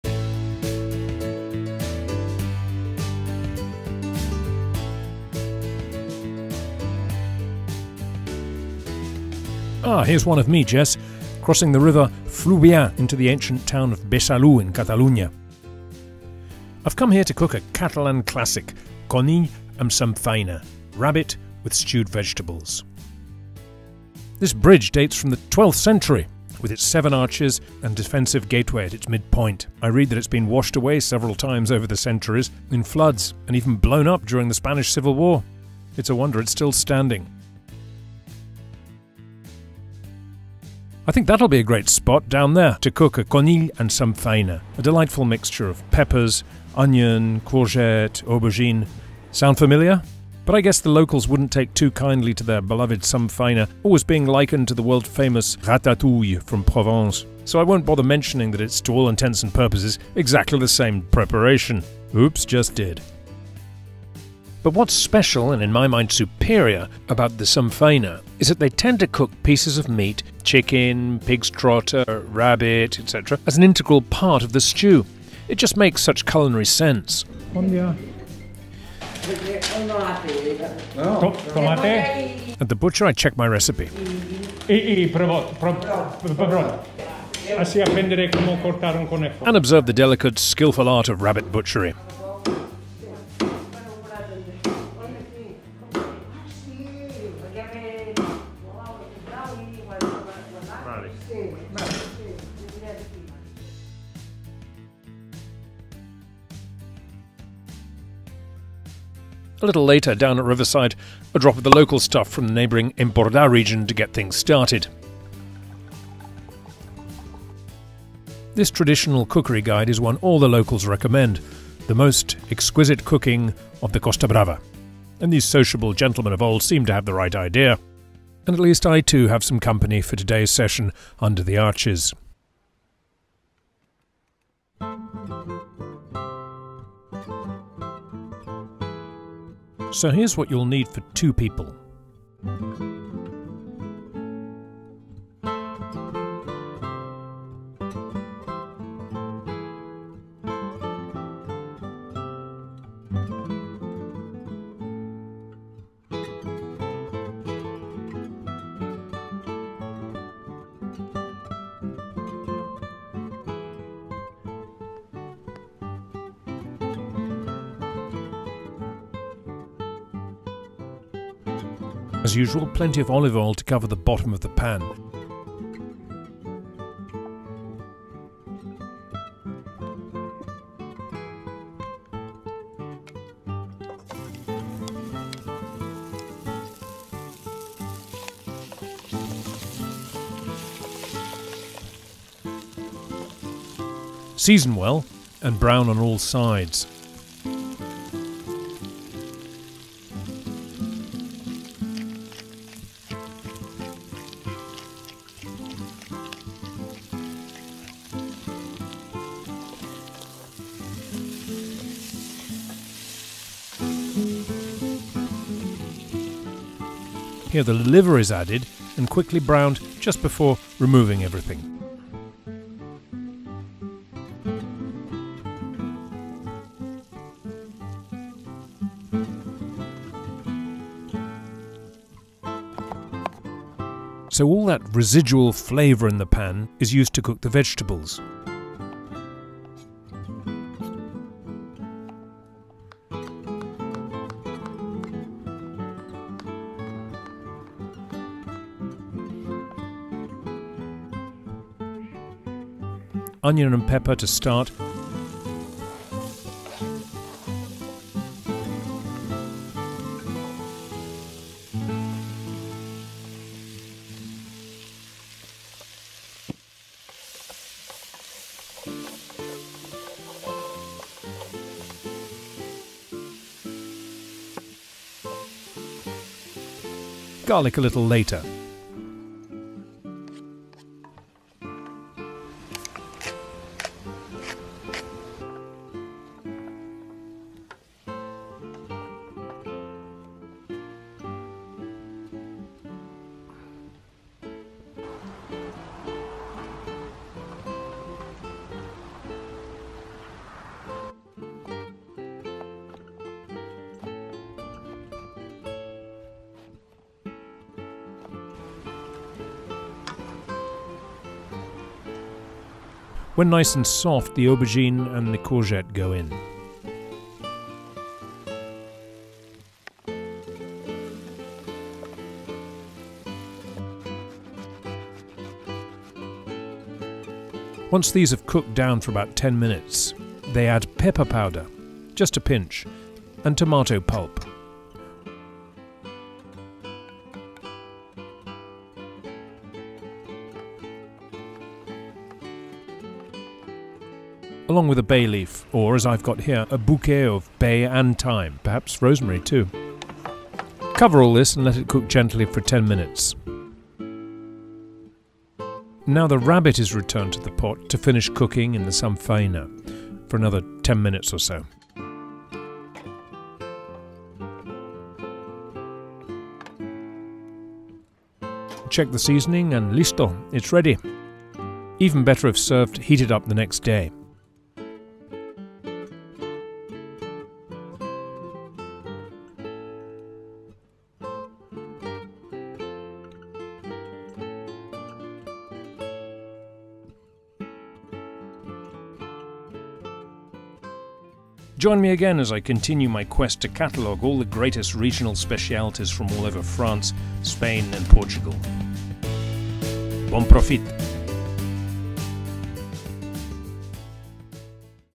The Classic Catalan Stew of Rabbit and Vegetables - SAMFAINA AMB CONILL
SAMFAINA AMB CONILL is a traditional Catalan stew of rabbit and various vegetables.